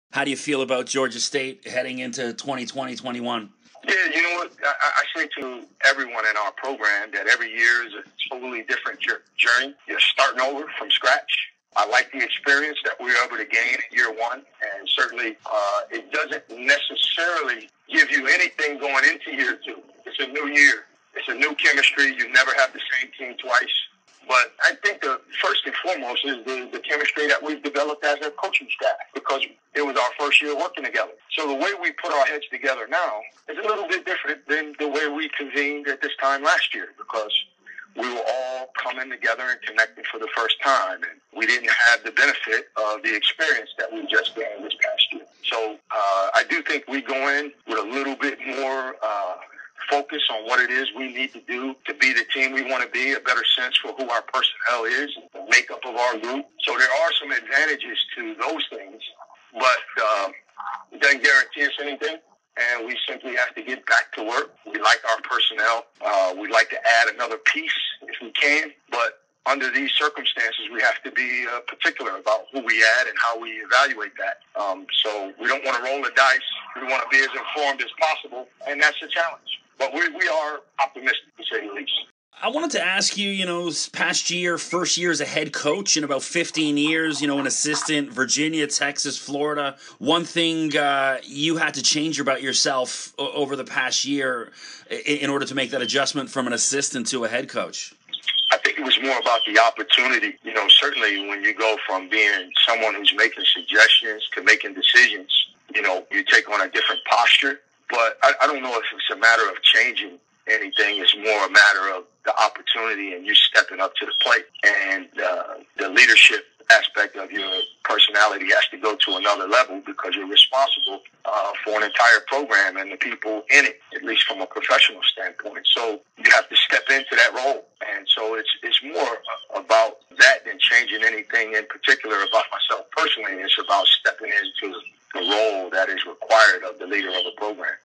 Rob Lanier on the future of Georgia State basketball in 2020-21 and his transition to head coach
Rob Lanier on the future of Georgia State basketball in 2020-21 and his transition to head coach. Twitter @MRMCKEE